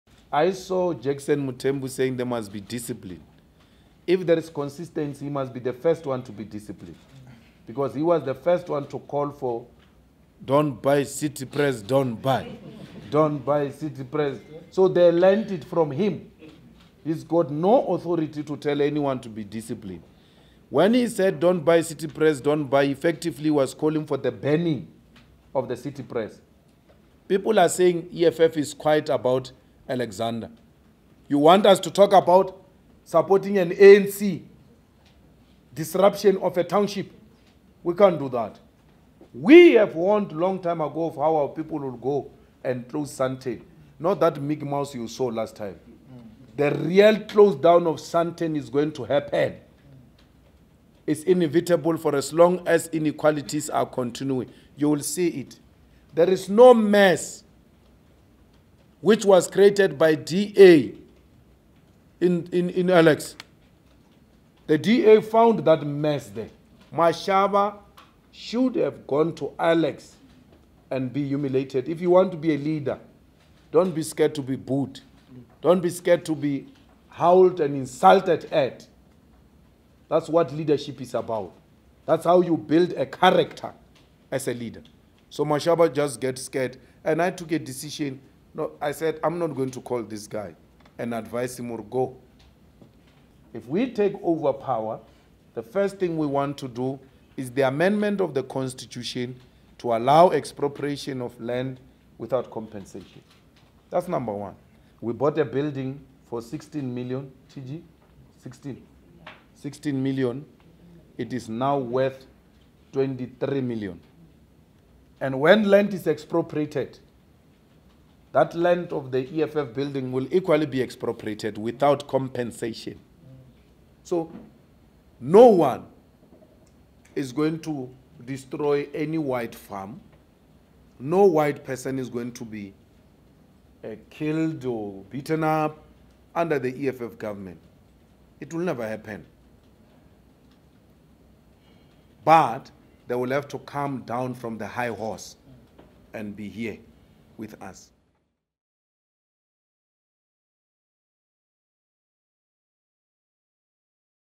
From ‘Gangster State’ protests to AlexTotalShutdown - top highlights from the EFF briefing
The EFF held a media briefing on April 10 2019 at which its leader, Julius Malema, highlighted a number of issues heading toward the elections, including the Alexandra protests and the disruption of Pieter-Louis Myburgh’s book launch.